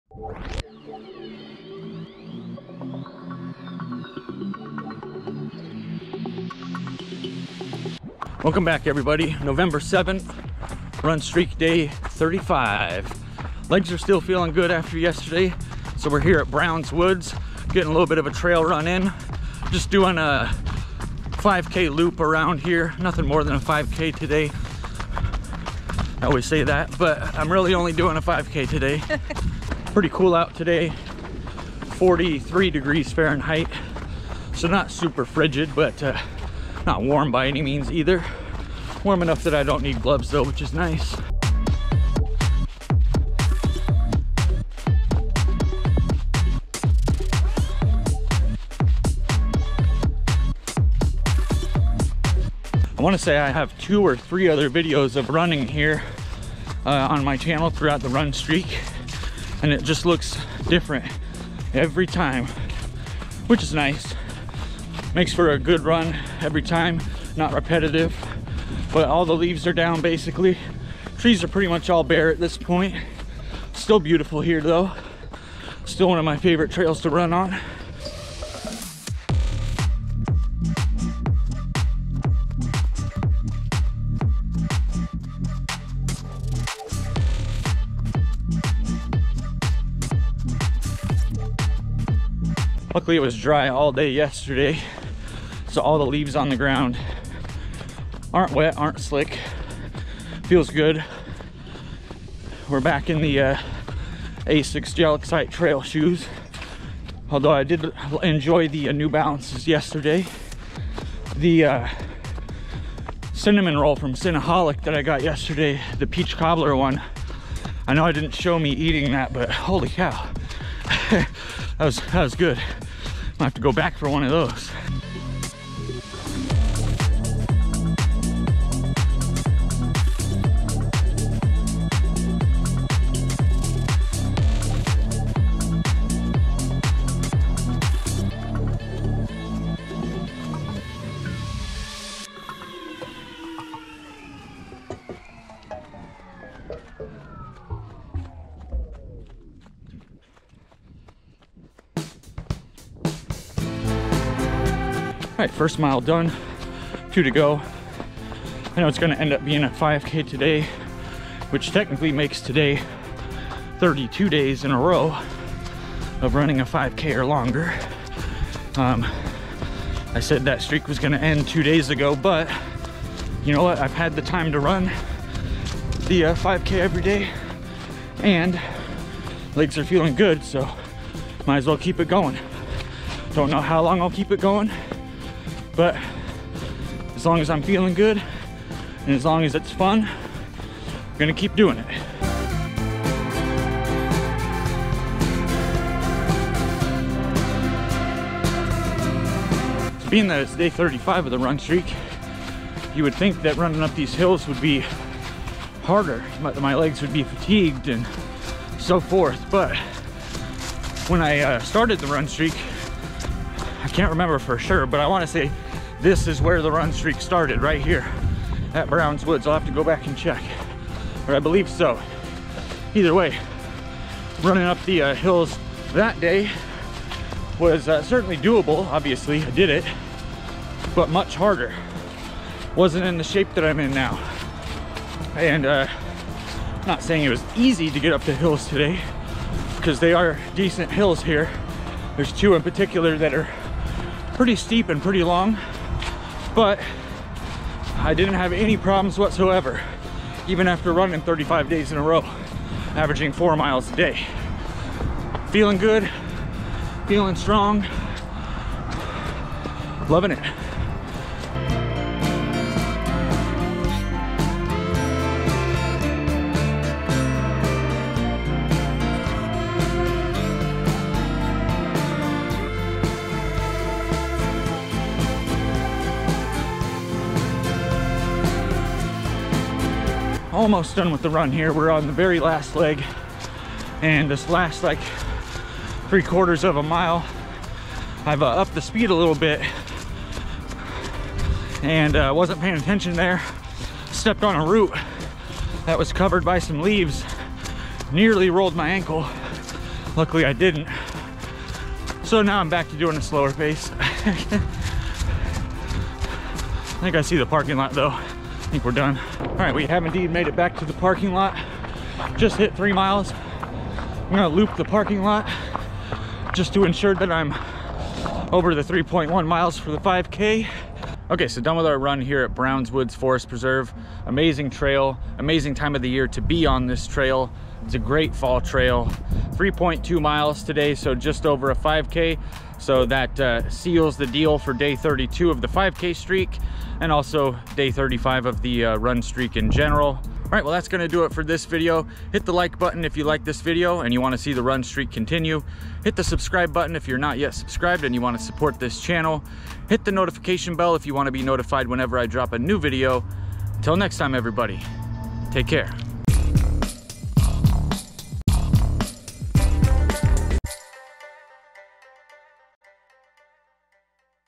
Run Streak Day 35 - 5k Streaking on Brown's Woods Trail - November 7th, 2022
In today's video, I head back to Brown's Woods Forest Preserve in West Des Moines for a 5K trail running session. This is day 35 of my current Run Streak and day 32 in a row of running a 5K or longer.

-- Chapters --
Welcome back: 0:07
Bridge Crossing: 1:19
First Mile Done: 2:34
Nearly Done: 4:39
Run Recap: 5:20